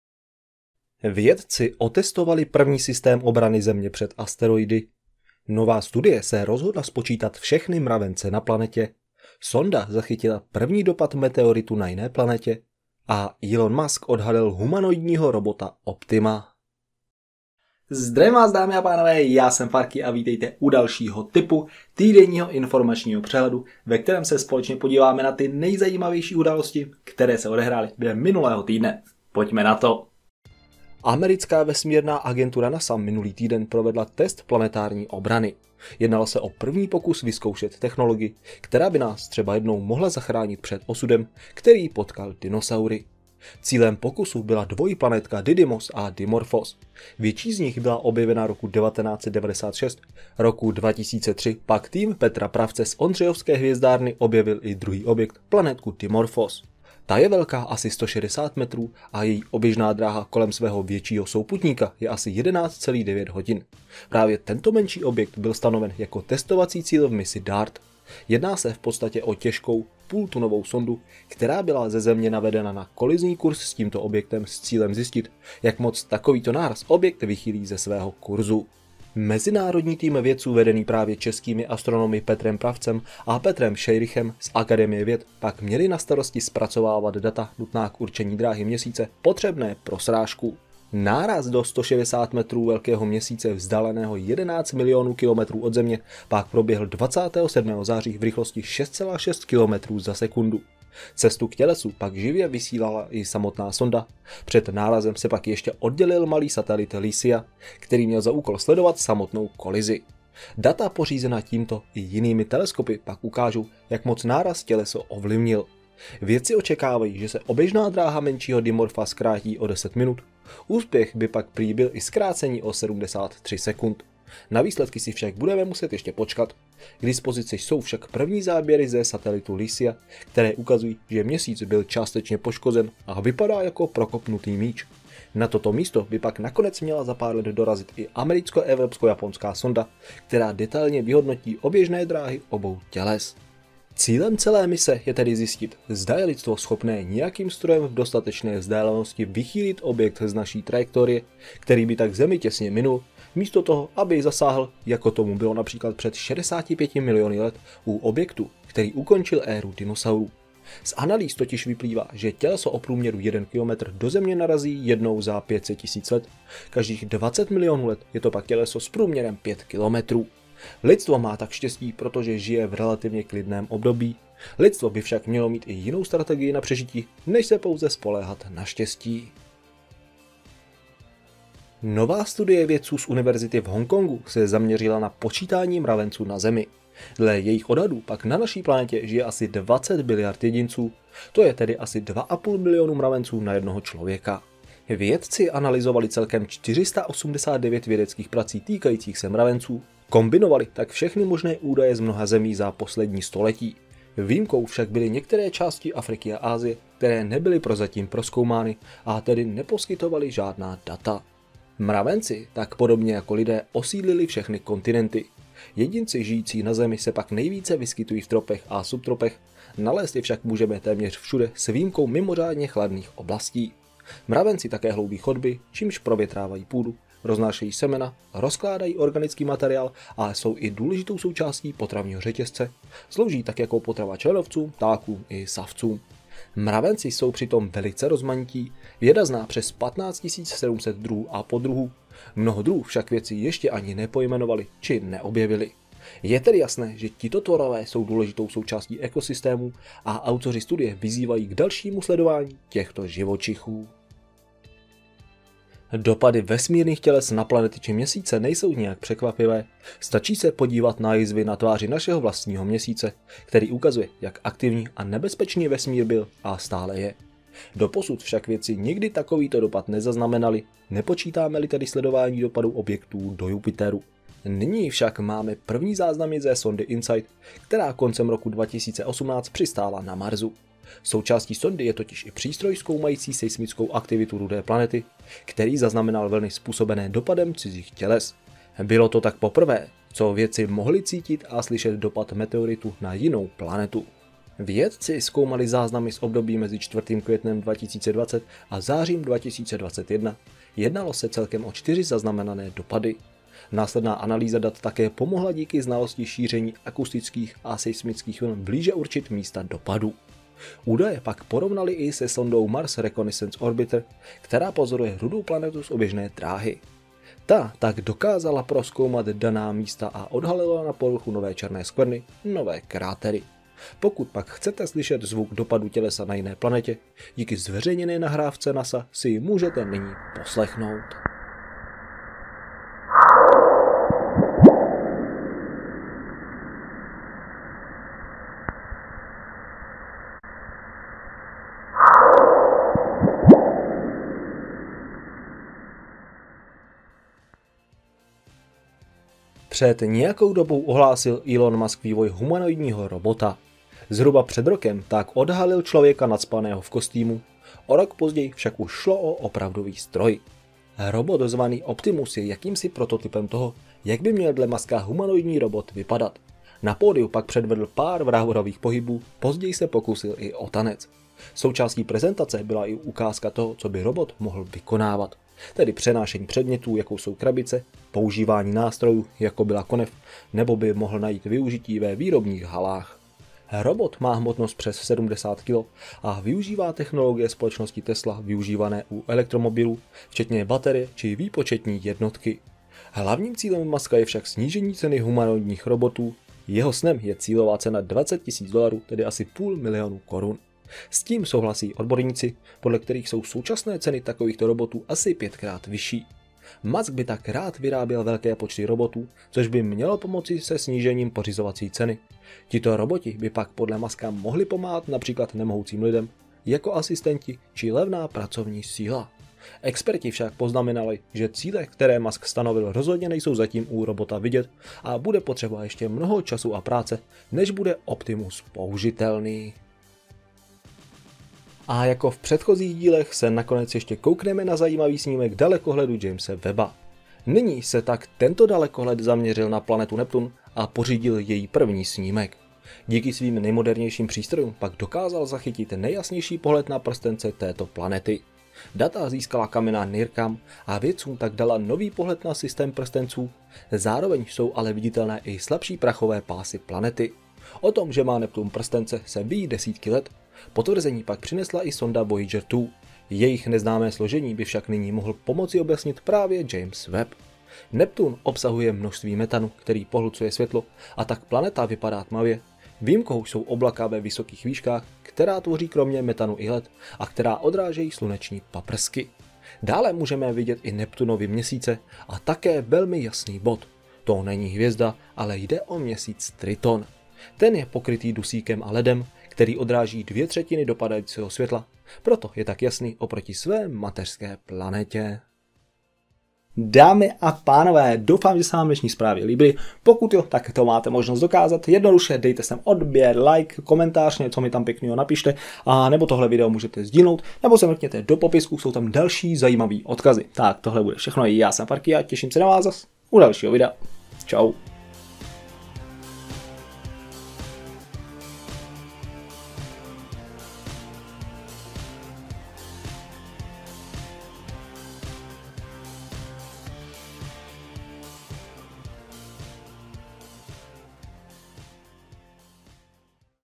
Kolik je na Zemi mravenců? Odklonění asteroidu/ Elon Musk Tesla robot Optimus [TIP 323]
Pozitivní zprávy T.I.P. - dnešní témata jsou:
00:00 Úvod
00:30 První test obrany Země před asteroidy

03:00 Zemi dle nových odhadů obývá 20 biliard mravenců

04:15 První záznam dopadu meteoritu na jiné planetě

05:48 Odhalení prototypu humanoidního robota Optima

07:11 Detailní pohled na Neptun i jeho prstence

08:16 Závěr

Podpořit mě můžeš členstvím zde na Youtube či Patreonu, nebo kryptoměnou!
Kryptoměnou:
BTC: bc1qmepwklmvqp7g3p2ju9er0sc3clh2scars2wa7k
ETH: 0xd3c6dF43dc9aE91F6ADCCB07C3c6F9b66cF905DF